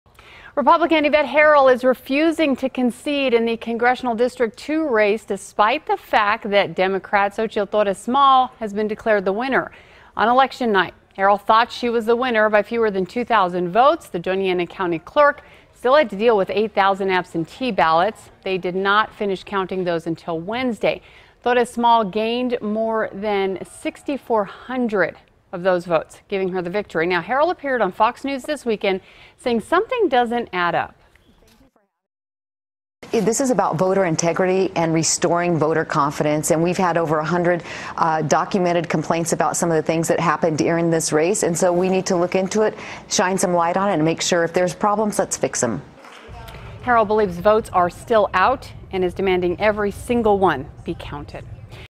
Yvette Herrell refuses to concede in 2nd Congressional District race
Yvette Herrell refuses to concede in 2nd Congressional District race - Source